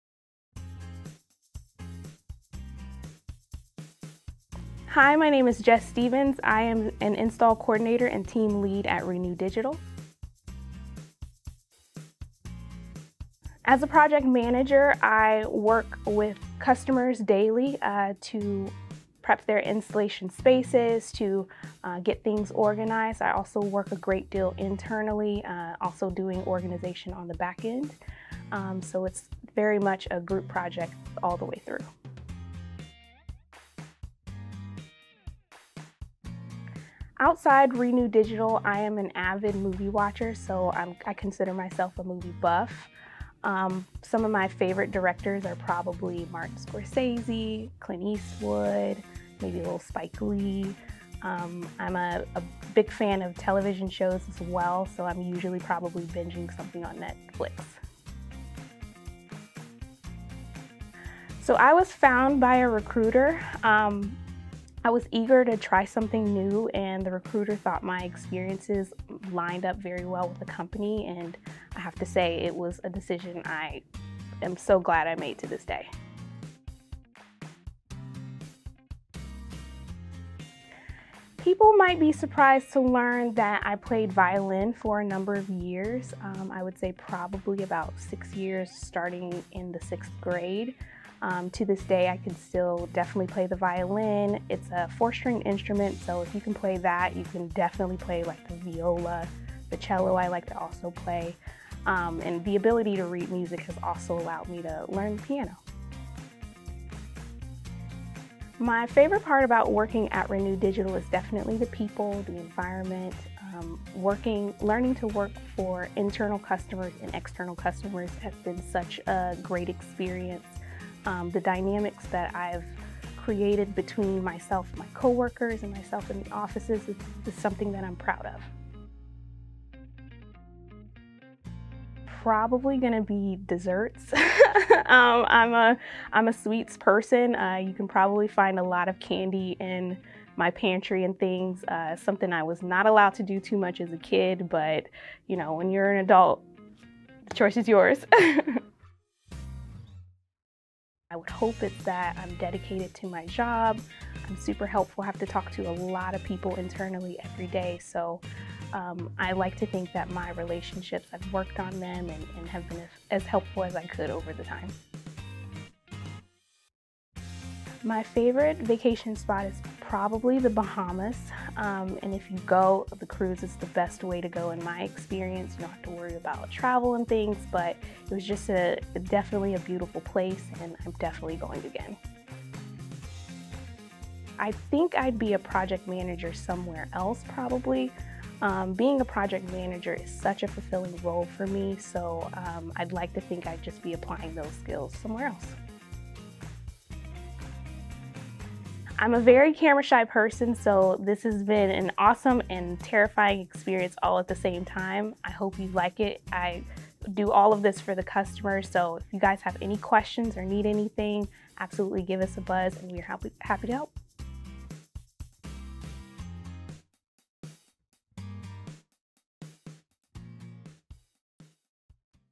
Renew Digital Staff Spotlight: Jess
Let's meet Jess, an Install Coordinator and Team Lead here at Renew Digital. An avid movie buff and violin player, who helps give our customers a five-star experience. Come see why she loves working with us!